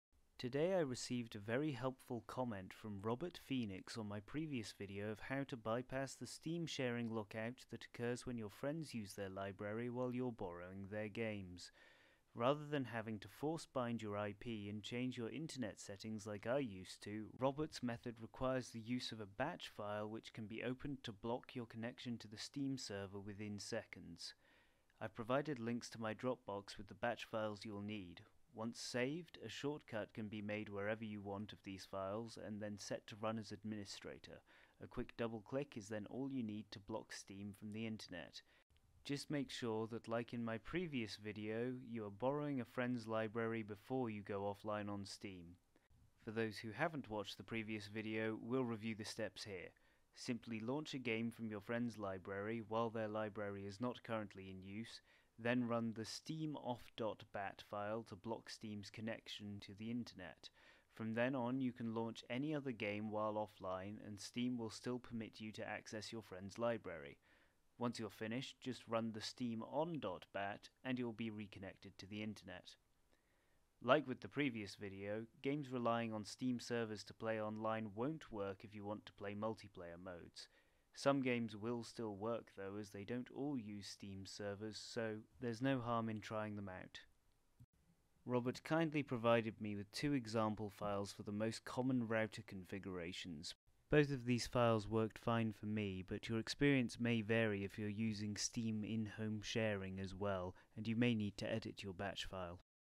Improved, simpler method for playing steam shared games while library is in use
A very helpful man showed me that things could be easier. This tutorial shows how to use the batch files linked in my dropbox to block steam's access to the internet and make it possible to continue playing steam games while offline but with a connection to the internet for other use.

If your steam application is not installed in its default directory, you will have to find where your steam.exe is and change the path in both your batch files.

I'll be honest, I don't really understand how this works but it seems simple enough. It's just switching on and off access through the firewall.

If anything goes wrong then go to windows defender/firewall and delete the firewall rules as such
In both the inbound and outbound rules.